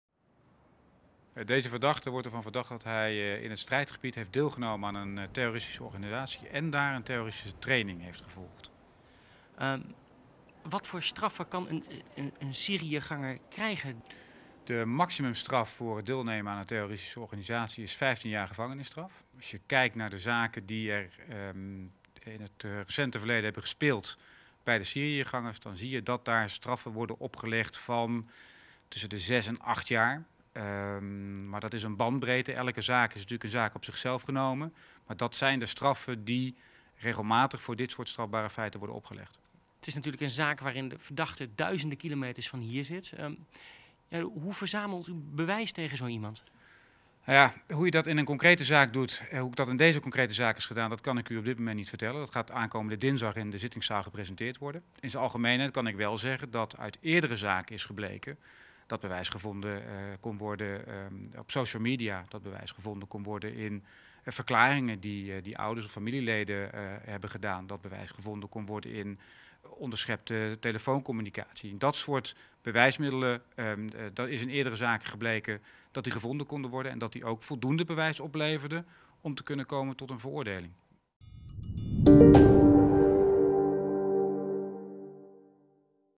Syriëganger Victor D. uit Heeten kan tot 15 jaar cel krijgen
Hij vertrok uit Heeten met een ideaal naar Syri, om te strijden tegen president Assad. Op de golf van de Arabische Lente sloot hij zich aan bij milities die later zouden worden aangemerkt als terreurorganisaties; Victor D. uit Heeten.